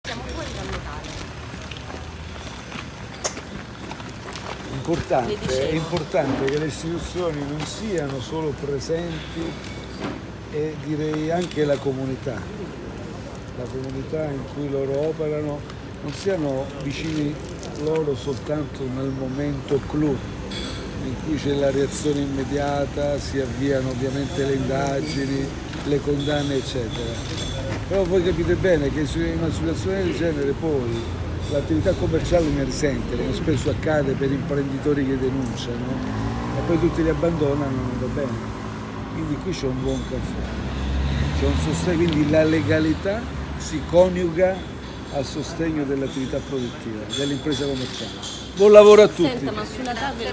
Conte visita il bar vittima del blitz dei Casamonica nel 2018
(Agenzia Vista) - Roma, 11 Marzo 2019 - Il Presidente del Consiglio Giuseppe Conte ha fatto visita al Roxy Bar nel quartiere La Romanina a Roma, per far visita all’attività commerciale vittima di un blitz dei Casamonica lo scorso anno.